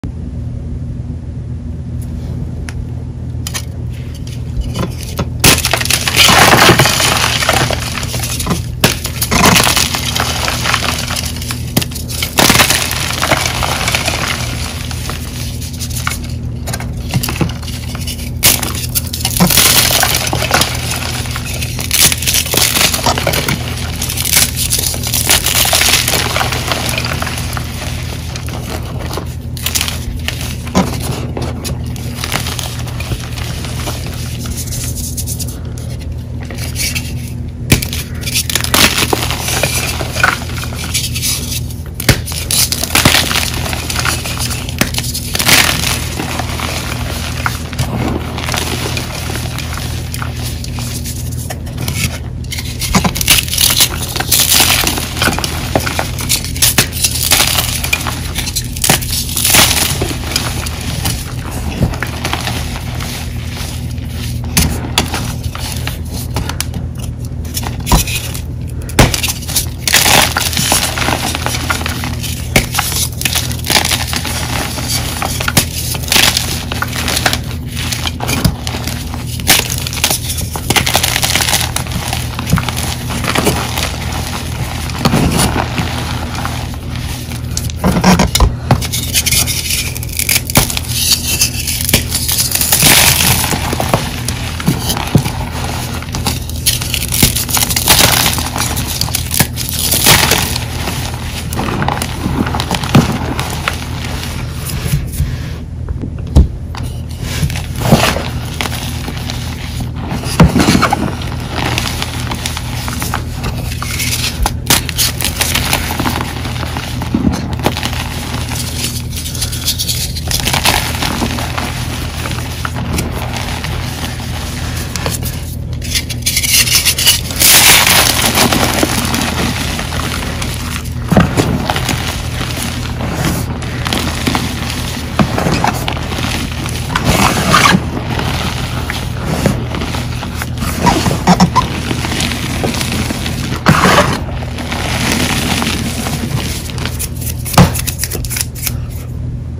Yellow + Purple Super Crispy Dusty Dyed Gym Crush
I was going to save this crush for a special occasion but I loved it so much that I felt like I wanted to share immediately. I also wanted to save It for after I post the not-so-great BSN crushes BUT I couldn’t wait! 😂😂🤷🏾‍♀️

There is something about yellow that makes the chalk EXTRA crispy! Idk what it is but it’s my favorite!